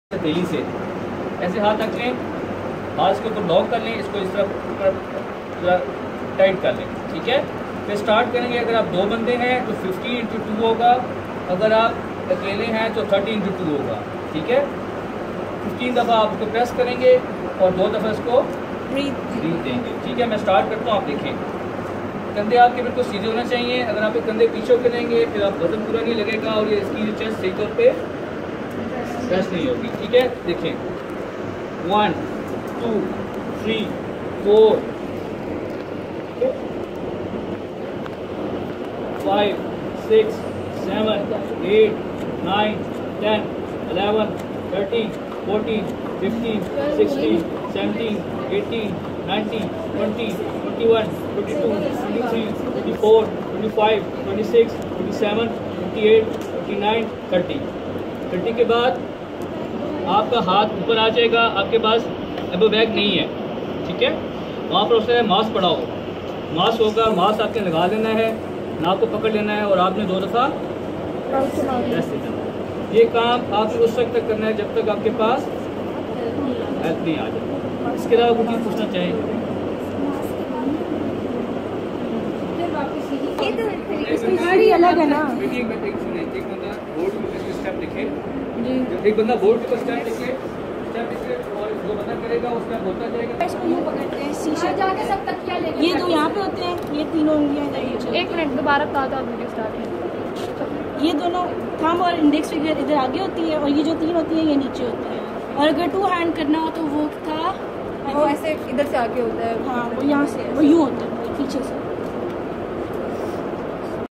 How to perform CPR.. BASIC LIFE SUPPORT
video showing the technique to do CPR. it is done in a patient of cardiac arrest.